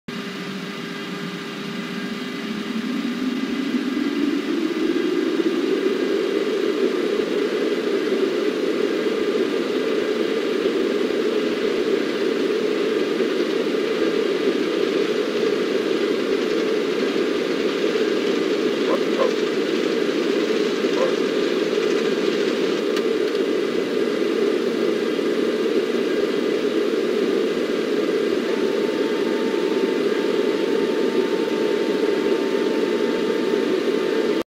John Glenn Columbus Intl Departure
Alaska Airlines Boeing 737-400 takes off.